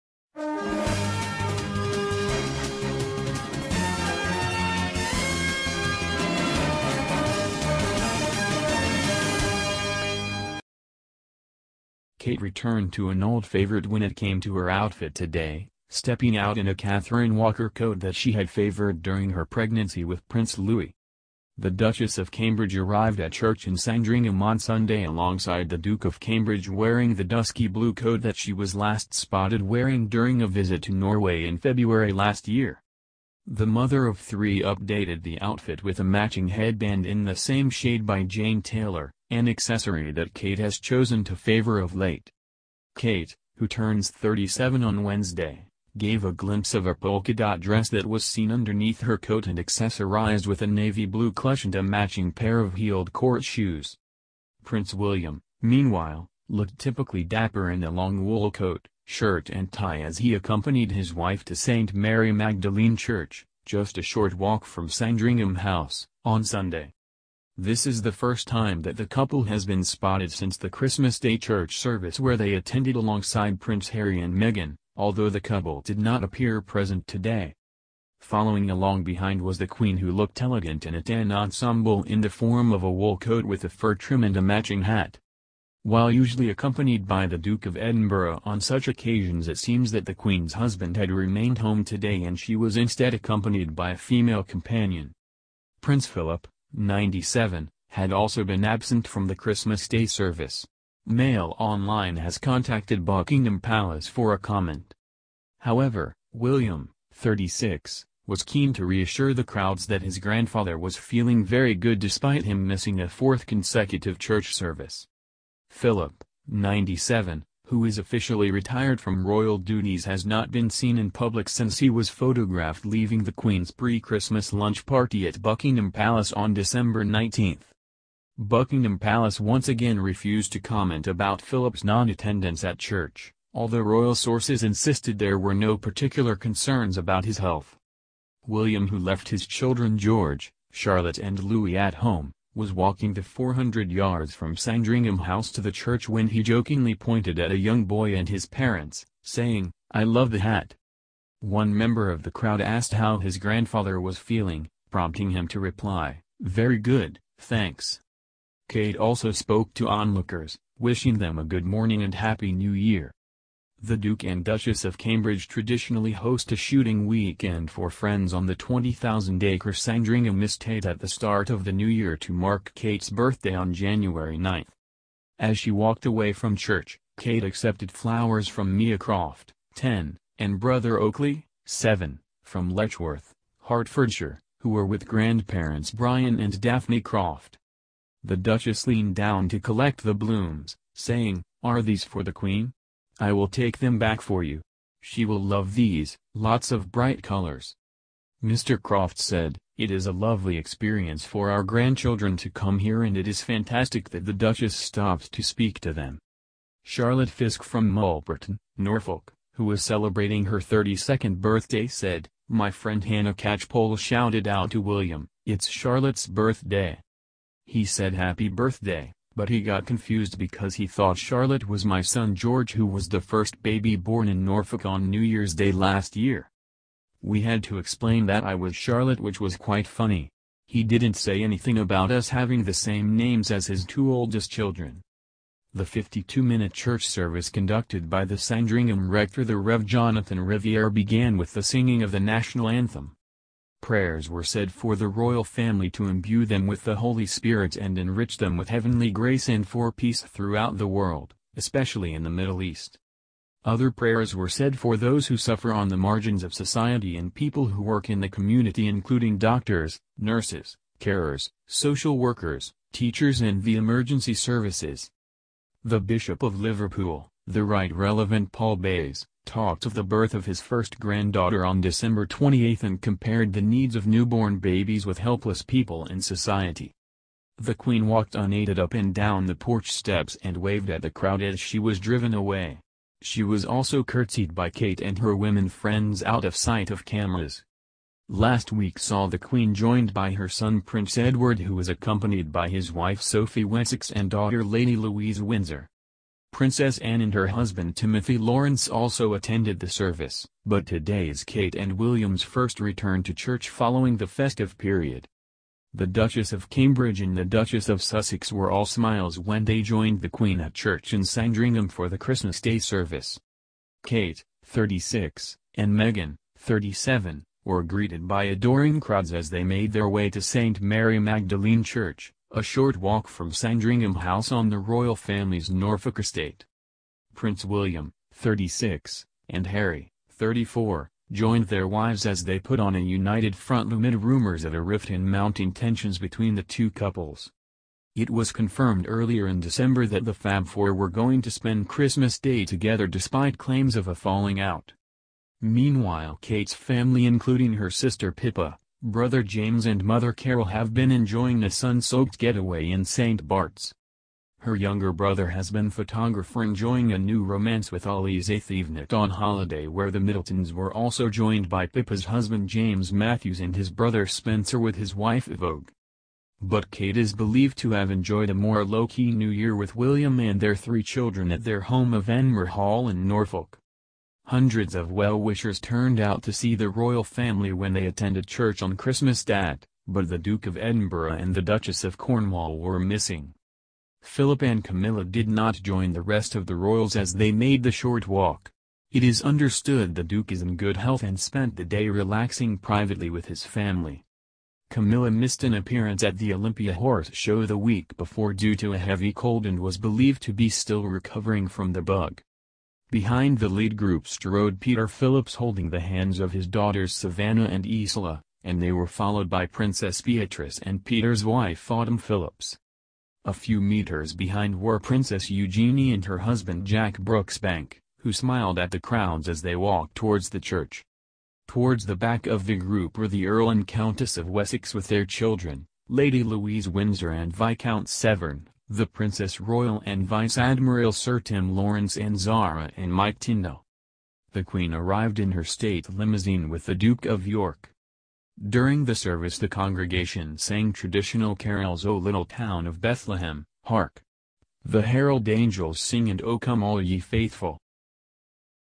Royal Thrifty: Kate Middleton re-wears £3000 Catherine Walker old coat to Sandringham church service
Kate goes blue with cold! Duchess wraps up in Catherine Walker coat she last wore in Norway to protect against the bitter weather as she and William join the Queen at church in Sandringham